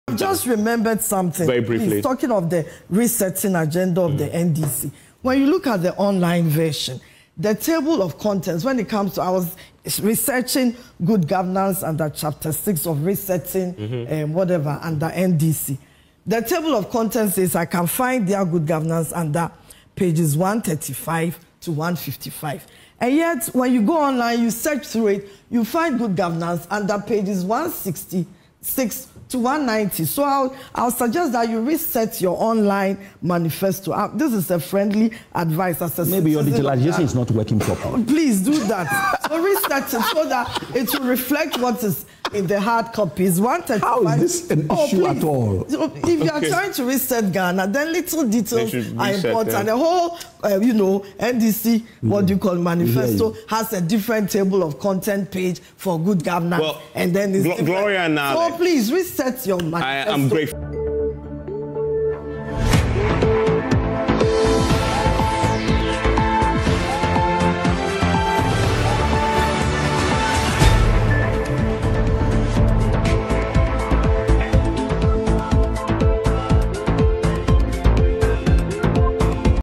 Gloria Boadu advises the NDC to reset their online manifesto to reflect what's in the hard copy.
Manifesto Tracker: Gloria Boadu advises the NDC to reset their online manifesto to reflect what's in the hard copy.